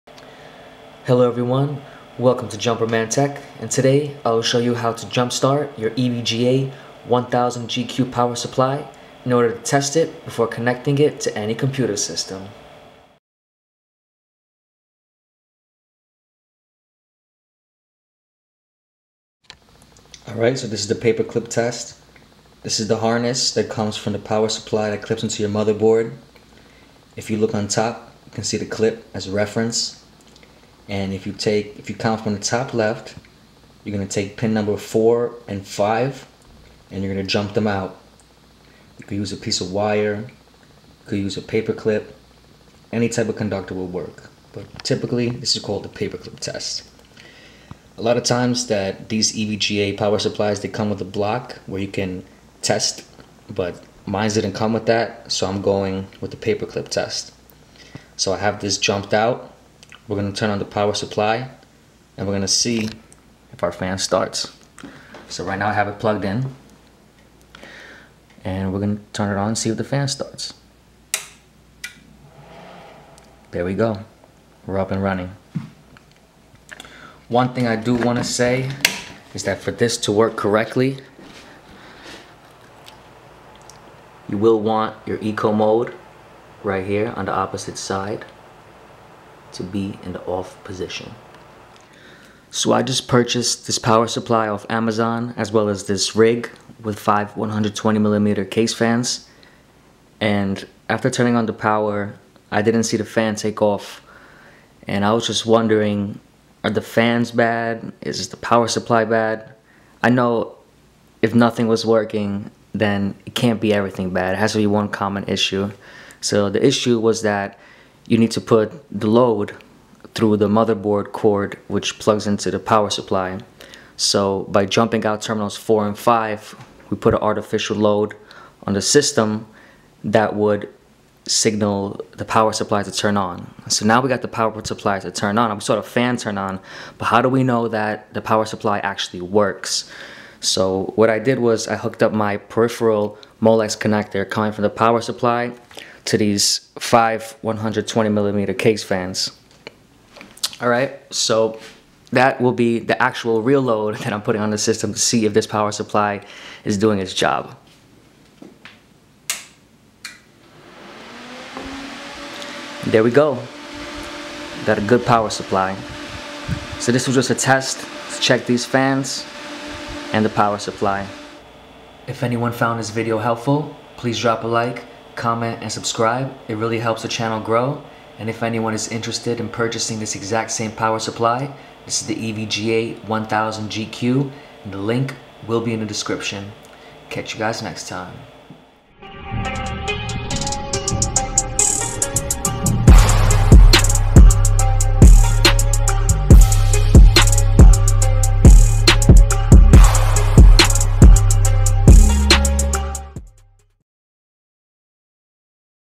How to test & jump start PC power supply (EVGA 1000 Paper clip test)
How to test & jump start my PC PSU power supply on its own without any motherboard or load

EVGA 1000 GQ Power Supply